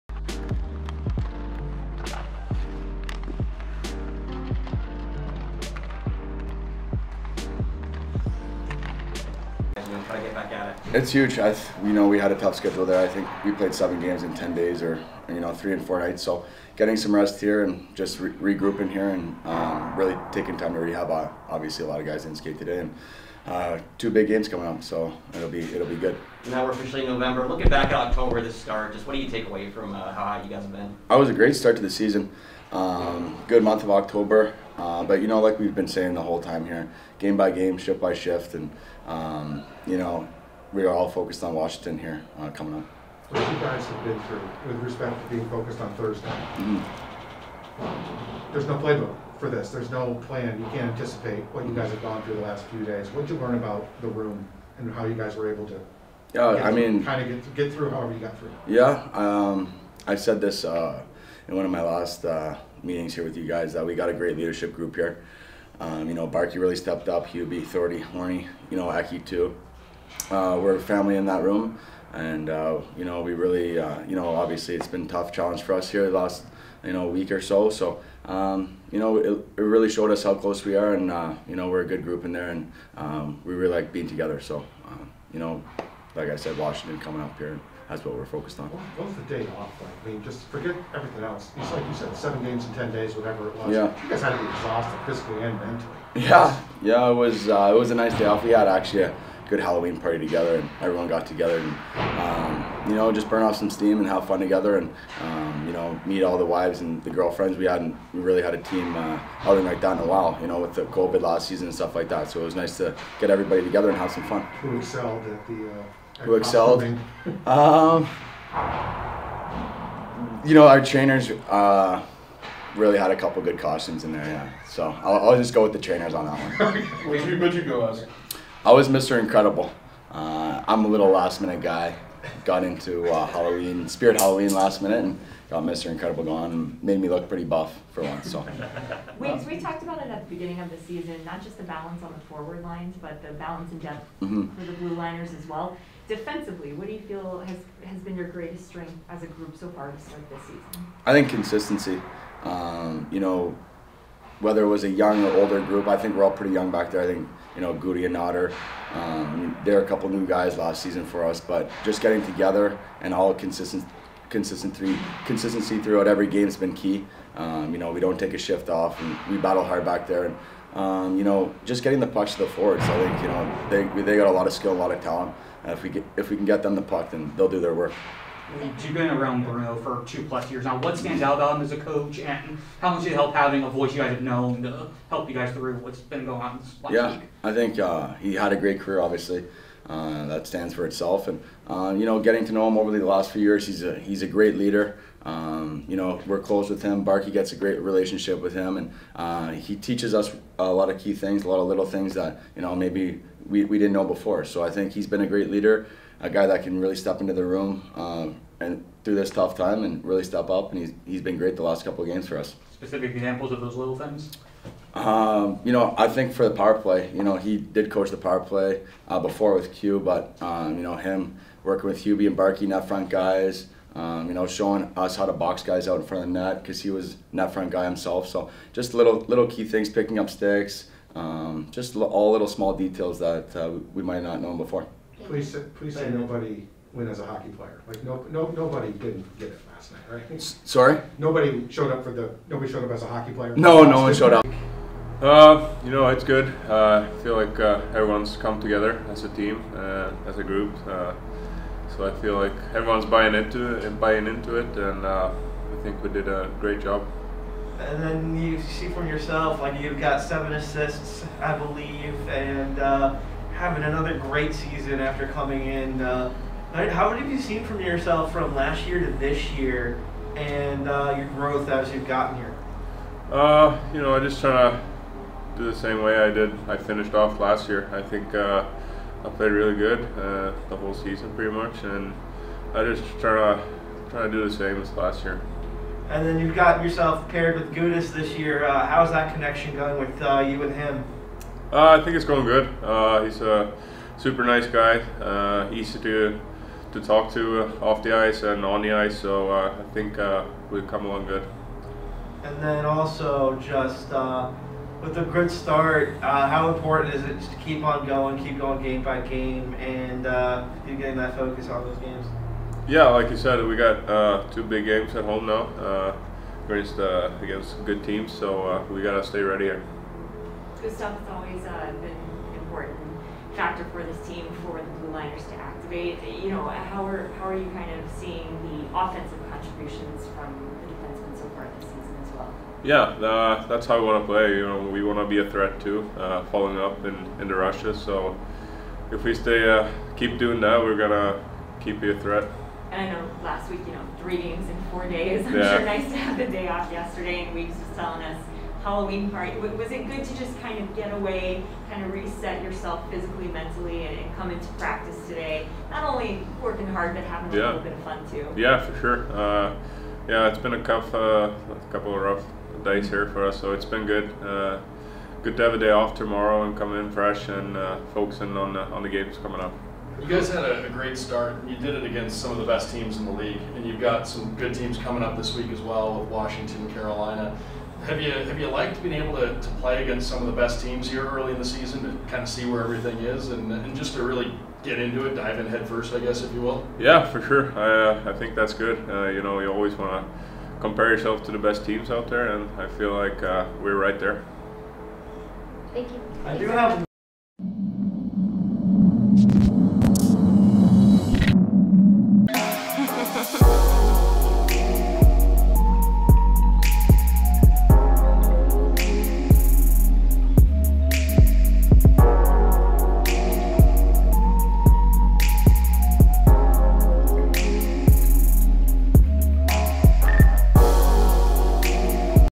MacKenzie Weegar & Gustav Forsling Detail Florida Panthers Adjusting After Quenneville Resignation
Watch as MacKenzie Weegar & Gustav Forsling Detail Florida Panthers Adjusting After Quenneville Resignation